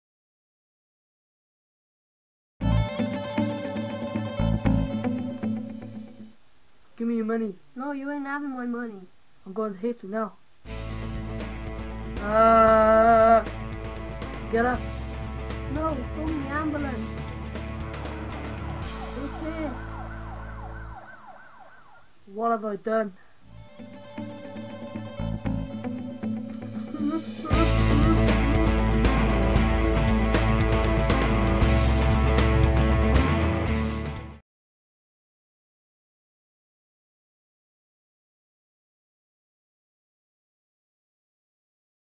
Bully - Animation
This animation about bullying was made by young people in Cardiff for theSprout.co.uk website.

TheSprout is Cardiff's youth website. To get involved check out